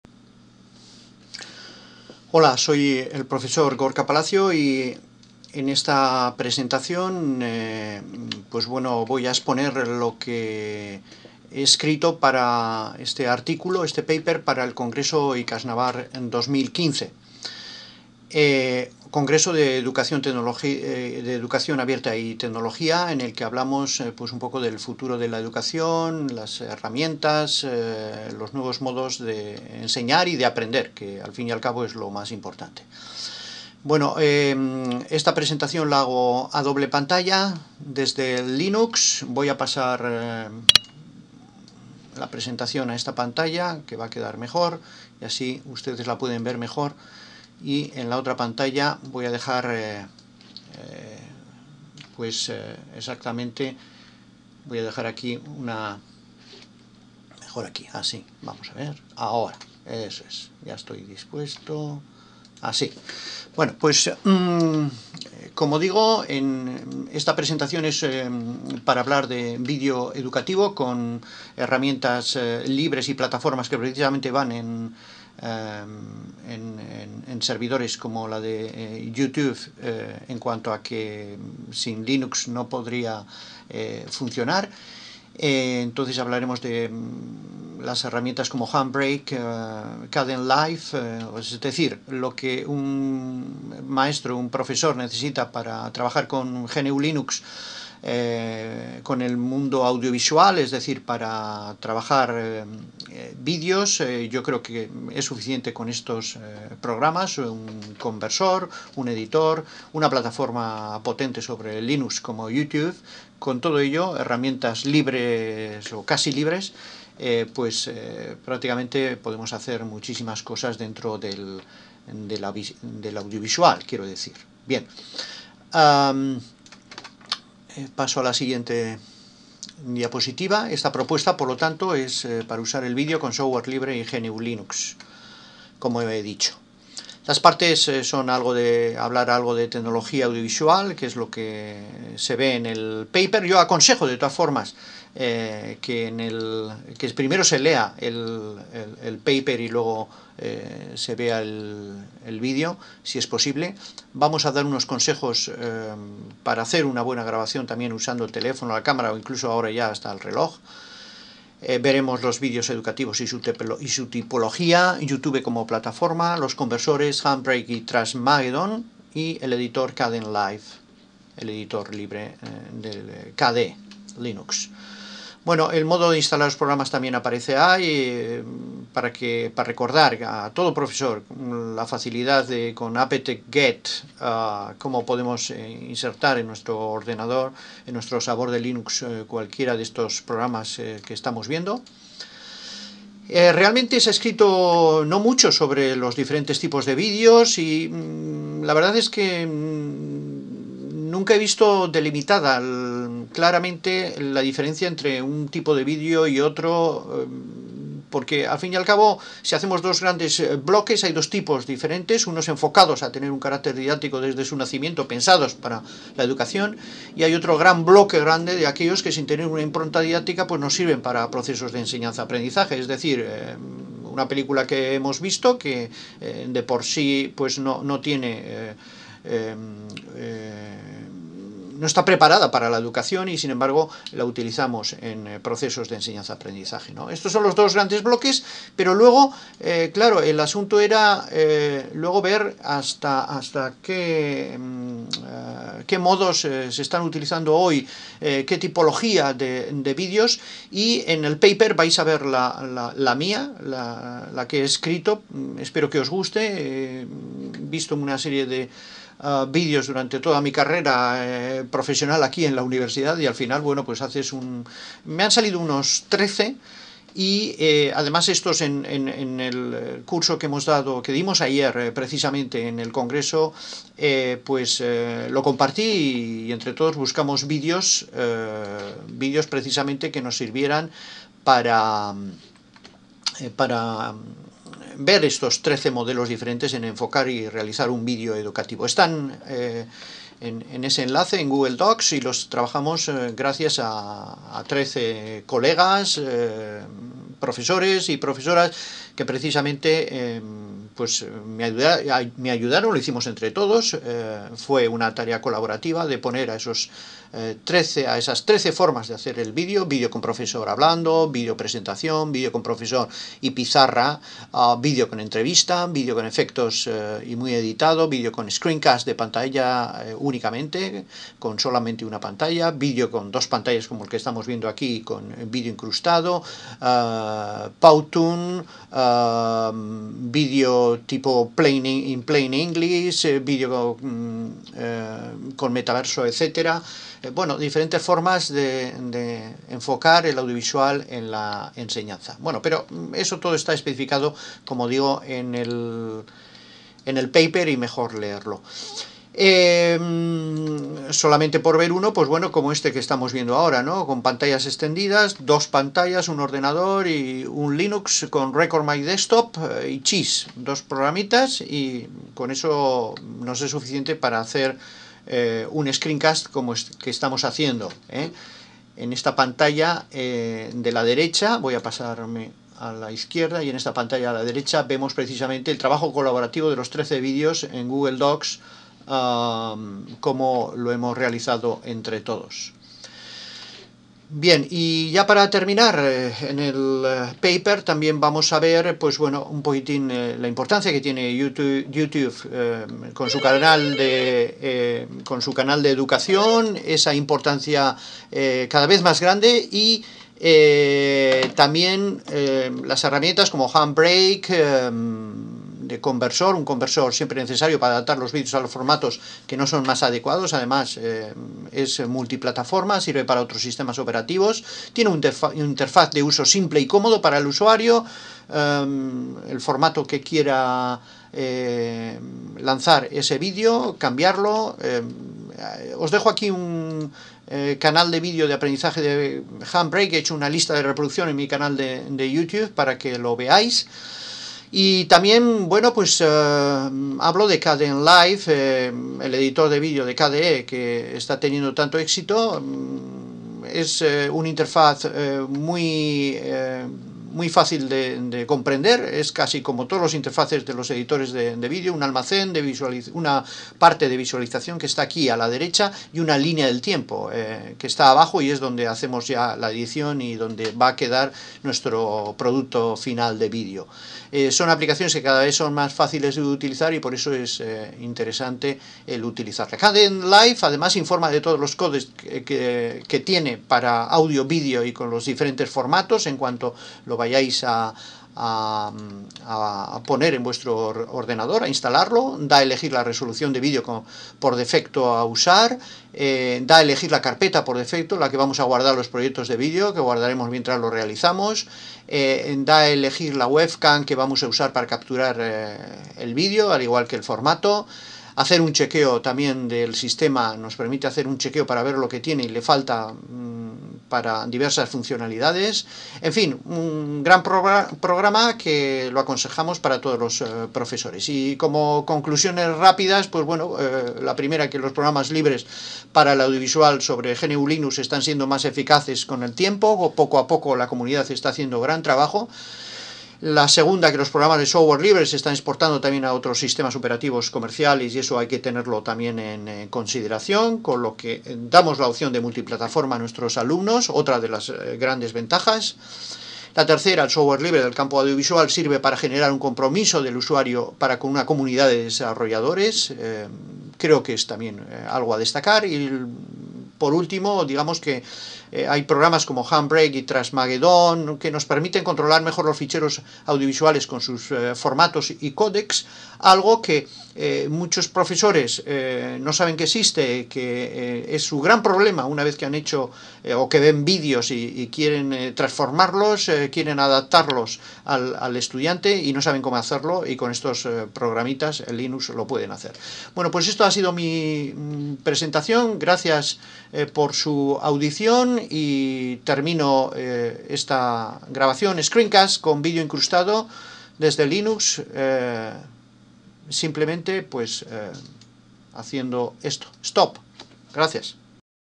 Herramientas libres de vídeo para la Educación
Vídeo con GNU/Linux, Kdenlive, Handbrake y Youtube como plataforma. Esa es la propuesta para el profesor y para el estudiante en los procesos de enseñanza-aprendizaje